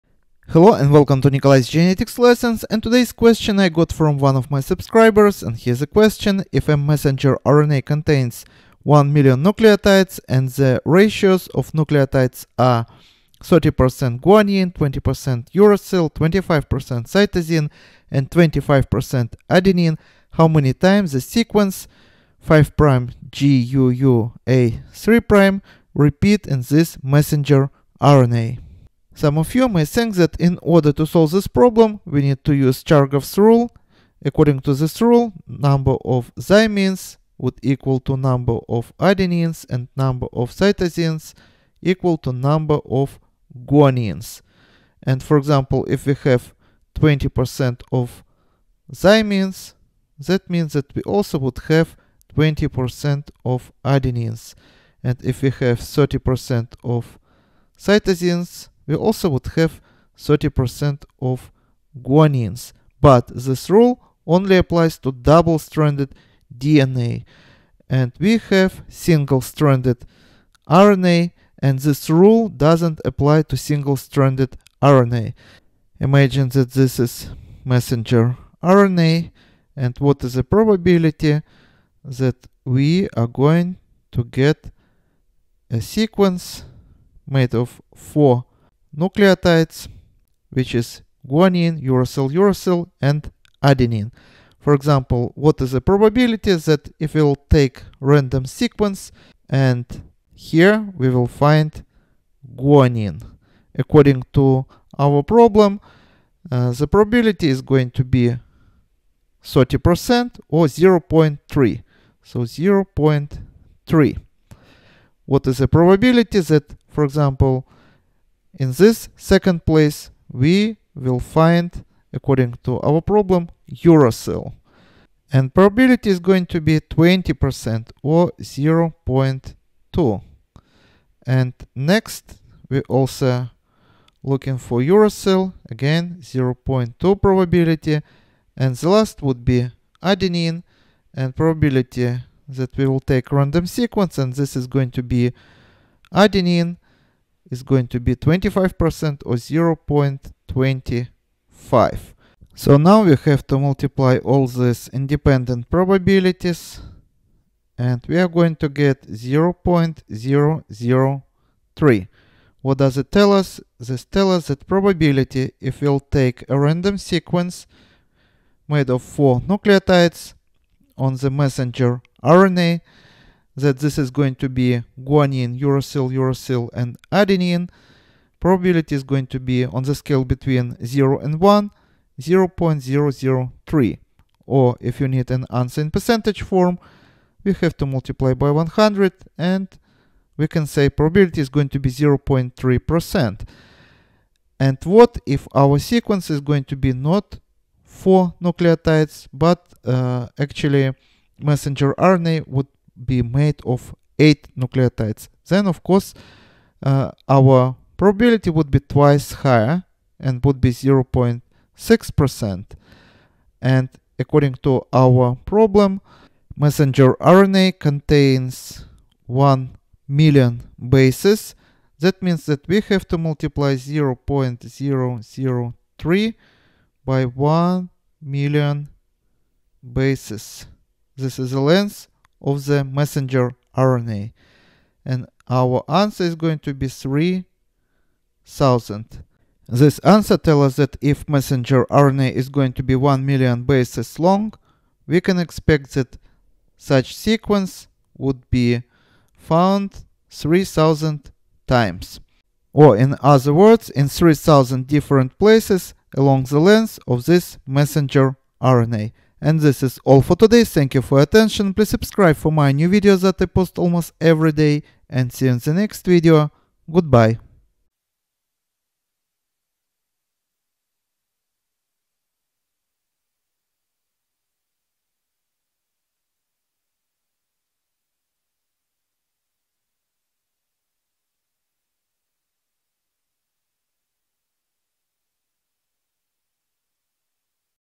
How to calculate probability of the sequence in a mRNA
The probability of two independent events occurring simultaneously is the product of the individual probabilities. The rule stating that the probability of the occurrence of independent events is the product of their separate probabilities.